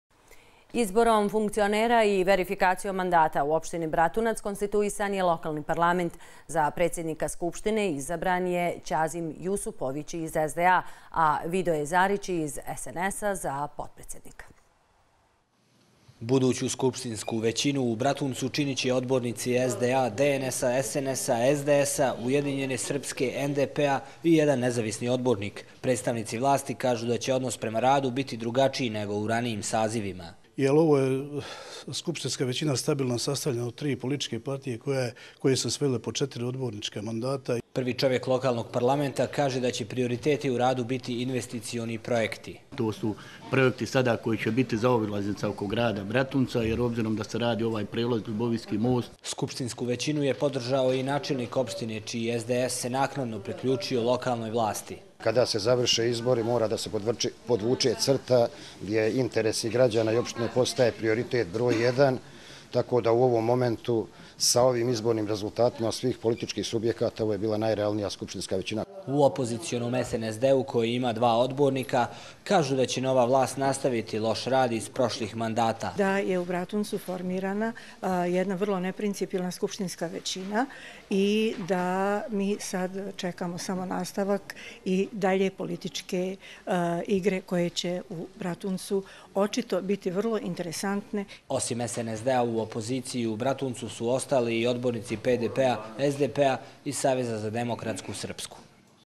Bratunac- Konstituisan lokalni parlament, DNS prekršio koalicioni sporazum
Izborom funkcionera i verifikacijom mandata, u opštini Bratunac konstituisan je novi saziv lokalnog parlamenta. Za predsjednika SO izabran je Ćazim Јusupović iz SDA, a Vidoje Zarić iz SNS-a za potpredsjednika.